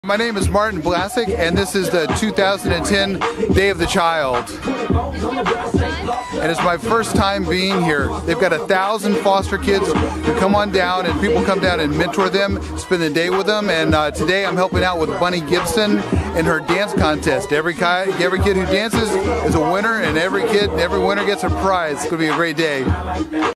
Martin Blasick at 2010 Day Of The Child
Martin Blasick mentored at the 2010 Day Of The Child. He helped original American Bandstand dancer Bunny Gilbson with her dance contest.